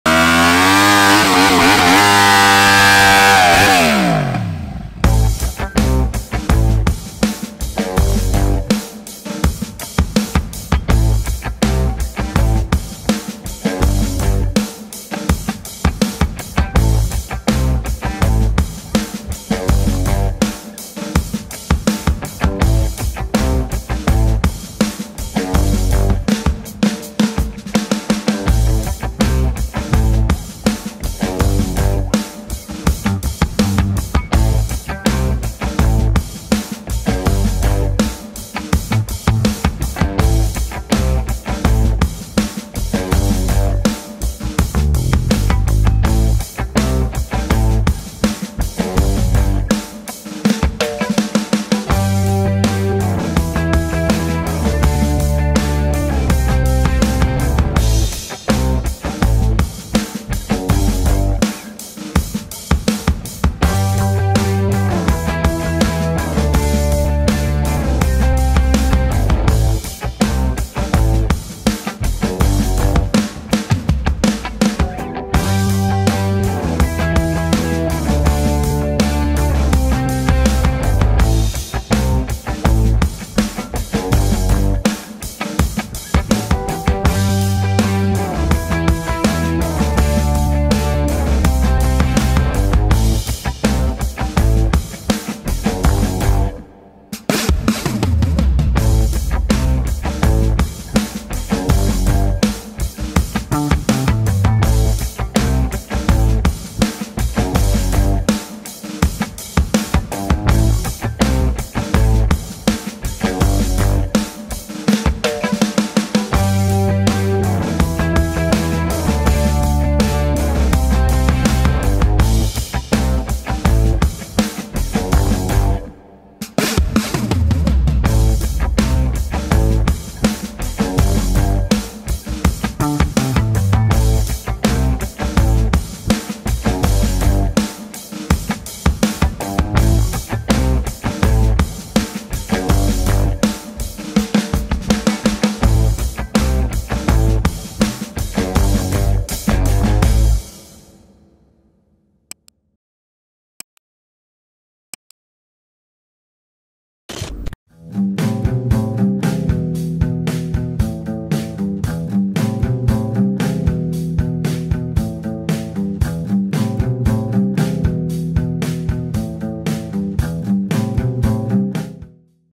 10 Motorsikal Paling Hot dan Terbaik 2022
Bosan dengan Motor lama dan nak tukar yang baru? Jom tengok 10 motorsikal Terbaik dan paling Hot tahun 2022.